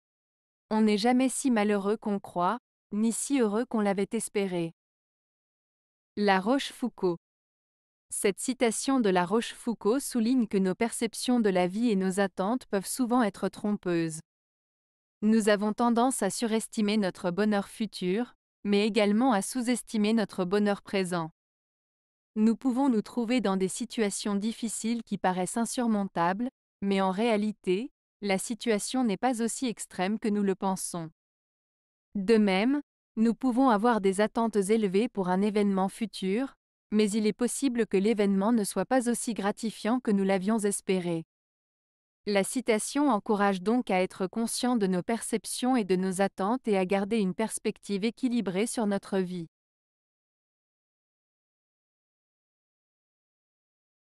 On n'est jamais si malheureux qu'on croit, ni si heureux qu'on l'avait espéré. - La Rochefoucauld
Cette citation de La Rochefoucauld souligne que nos perceptions de la vie et nos attentes peuvent souvent être trompeuses. Nous avons tendance à surestimer notre bonheur futur, mais également à sous-estimer notre bonheur présent. Nous pouvons nous trouver dans des situations difficiles qui paraissent insurmontables, mais en réalité, la situation n'est pas aussi extrême que nous le pensons. De même, nous pouvons avoir des attentes élevées pour un événement futur, mais il est possible que l'événement ne soit pas aussi gratifiant que nous l'avions espéré. La citation encourage donc à être conscient de nos perceptions et de nos attentes et à garder une perspective équilibrée sur notre vie.